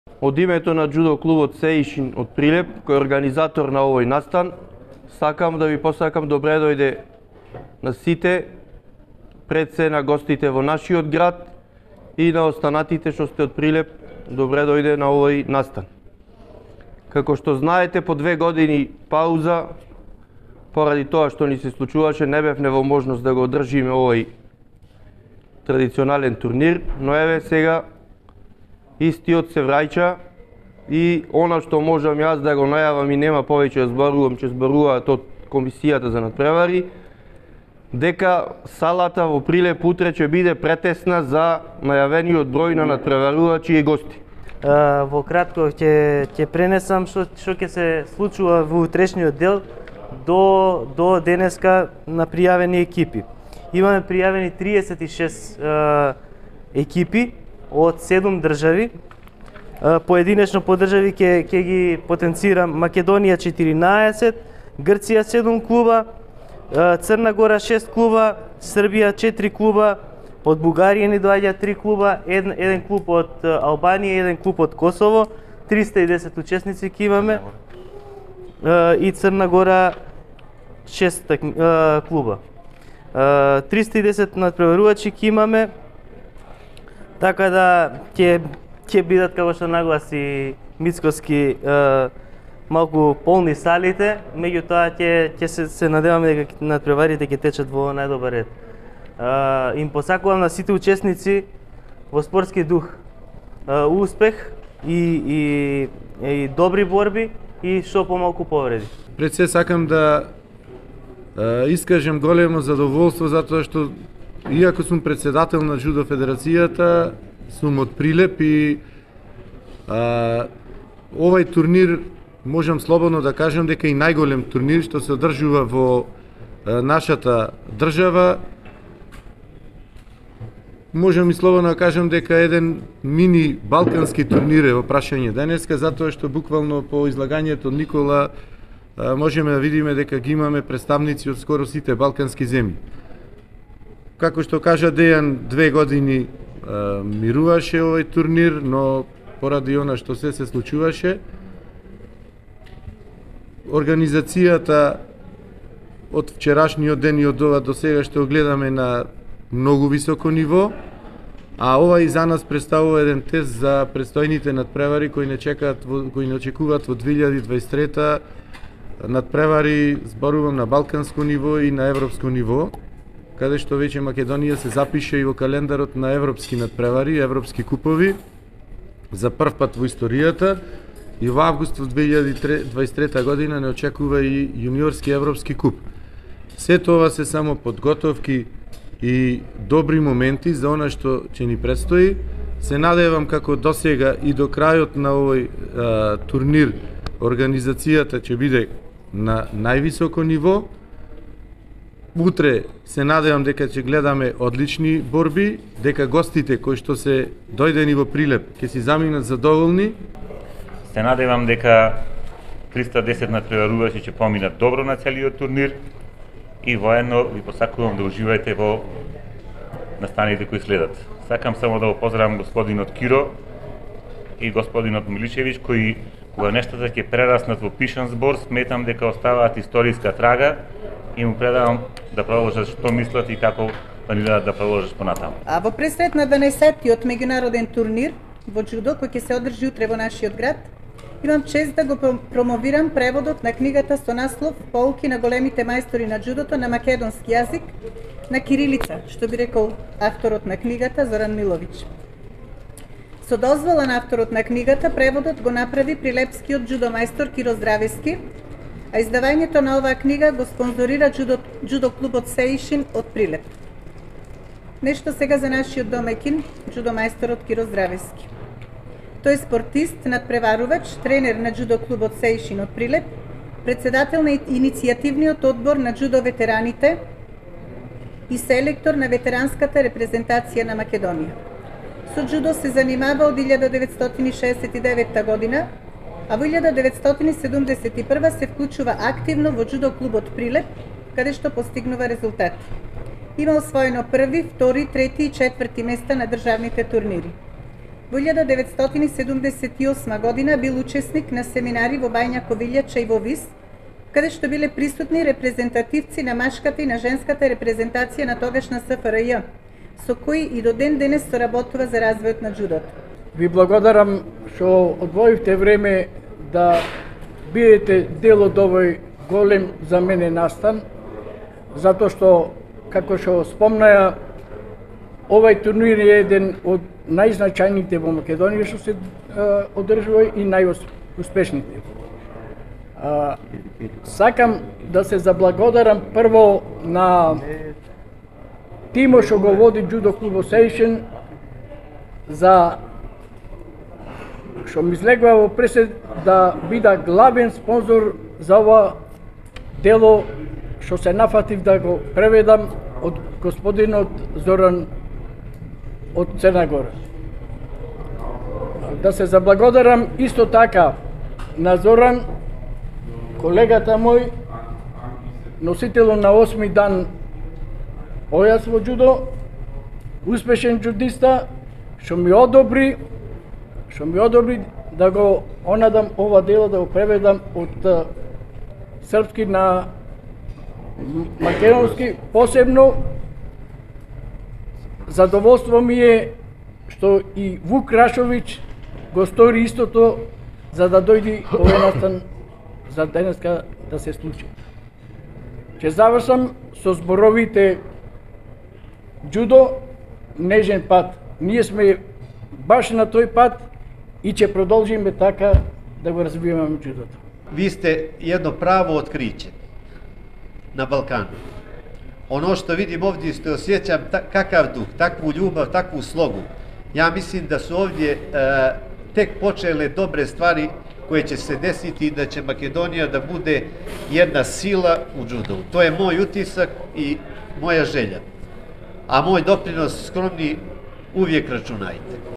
12 -ти интернационален џудо турнир Прилеп Град Херој и промоција на книга
На официјалната најава за одржување на 12-от интернацијонален џудо турнир Прилеп Град Херој беше промовирана и книгата Поуки на големите мајстори на џудото , преведена од прилепскиот џудо ветеран Киро Здравески